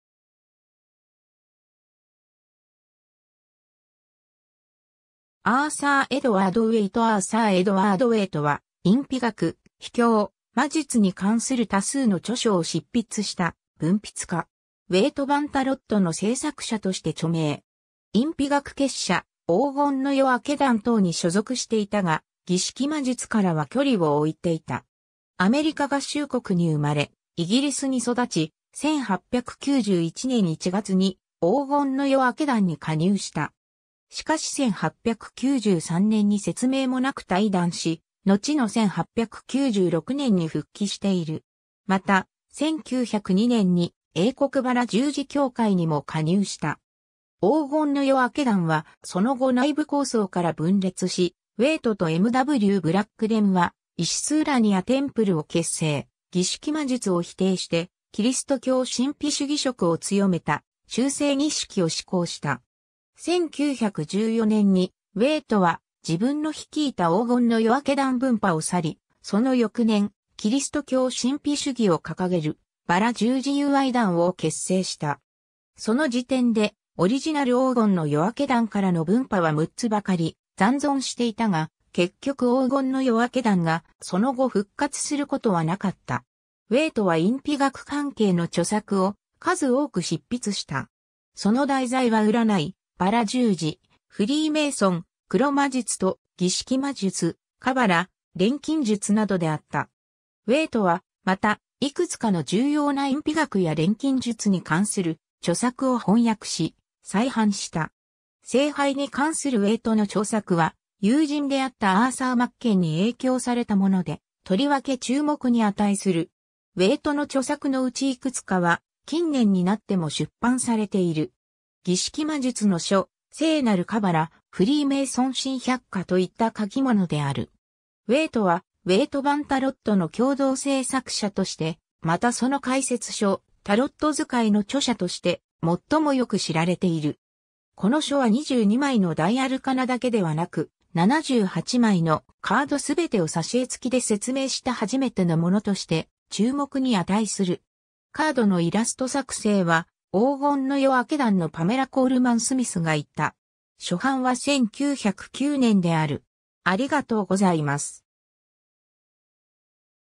アーサー・エドワード・ウェイト
Arthur Edward Waite アーサー・エドワード・ウェイト（Arthur Edward Waite、1857年10月2日 - 1942年5月19日）は、隠秘学、秘教、魔術に関する多数の著書を執筆した文筆家。
ウェイト版タロットの制作者として著名。
隠秘学結社「黄金の夜明け団」等に所属していたが、儀式魔術からは距離を置いていた。
 アメリカ合衆国に生まれイギリスに育ち、1891年1月に黄金の夜明け団に加入した。
しかし1893年に説明もなく退団し、のちの1896年に復帰している。
また、1902年に英国薔薇十字協会にも加入した。
黄金の夜明け団はその後内部抗争から分裂し、ウェイトとM・W・ブラックデンは（黄金の夜明け団創立時の最初のテンプルと同名の）イシス＝ウラニア・テンプルを結成、儀式魔術を否定してキリスト教神秘主義色を強めた修正儀式を施行した。
1914年にウェイトは自分の率いた黄金の夜明け団分派を去り、その翌年、キリスト教神秘主義を掲げる「薔薇十字友愛団」 (Fellowship of the Rosy Cross) を結成した。
その時点でオリジナル黄金の夜明け団からの分派は6つばかり残存していたが、結局黄金の夜明け団がその後復活することはなかった。
 ウェイトは隠秘学関係の著作を数多く執筆した。
その題材は占い、薔薇十字、フリーメイソン、黒魔術と儀式魔術、 カバラ、錬金術などであった。
ウェイトはまた、いくつかの重要な隠秘学や錬金術に関する著作を翻訳し、再版した。
聖杯に関するウェイトの著作は、友人であったアーサー・マッケンに影響されたもので、とりわけ注目に値する。
ウェイトの著作のうちいくつかは近年になっても出版されている。
『儀式魔術の書』 (The Book of Ceremonial Magic)、『聖なるカバラ』 (The Holy Kabbalah)、『フリーメーソン新百科』 (A New Encyclopedia of Freemasonry) といった書物である。
 ウェイトは、ウェイト版タロットの共同制作者として、またその解説書『タロット図解』 (The Pictorial Key to the Tarot) の著者として、最もよく知られている。
この書は22枚の大アルカナだけではなく、78枚のカード全てを挿絵付きで説明した初めてのものとして注目に値する。
カードのイラスト作製は黄金の夜明け団のパメラ・コールマン・スミスが行った。
初版は1909年である。